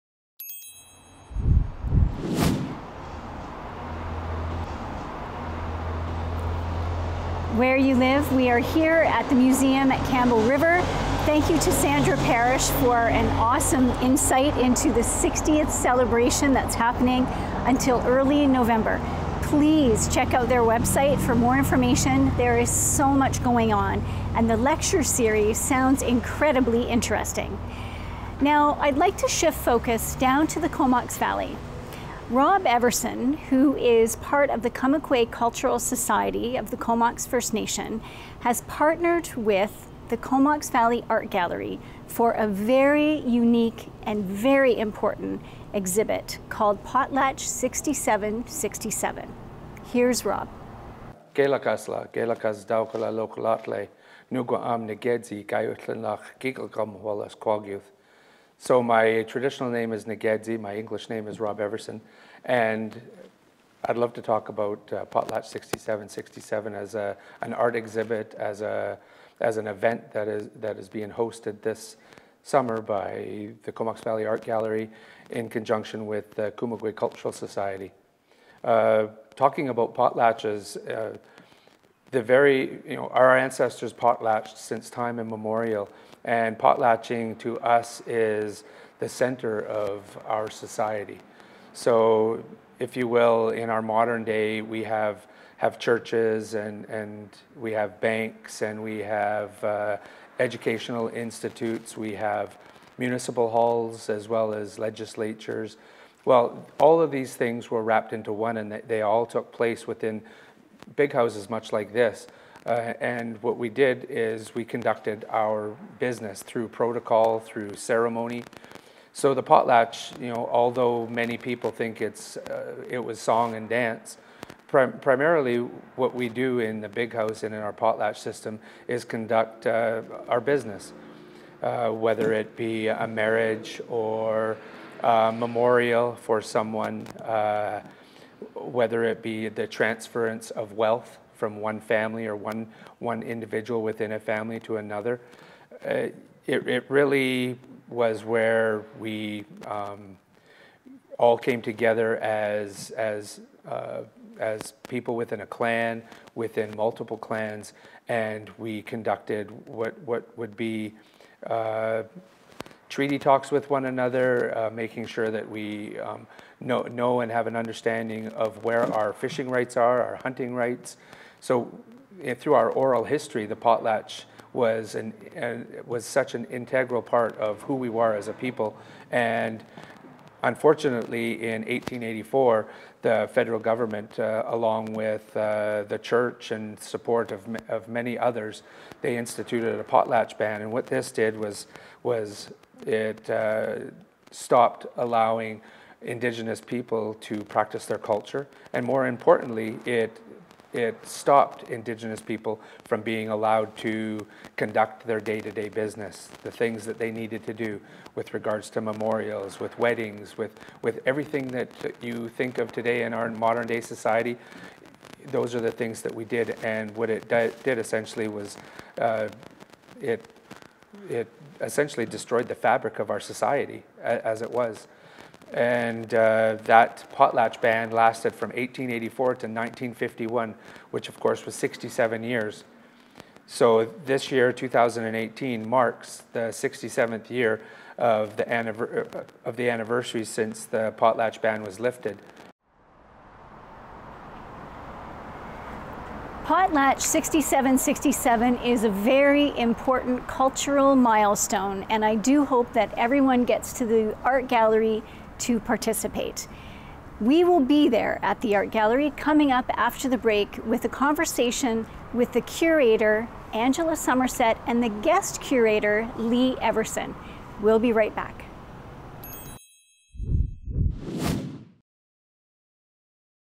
Potlatch 67-67 with Rob Everson
Rob Everson of the Kumugwe Cultural Society explains the history of the Potlatch Ban.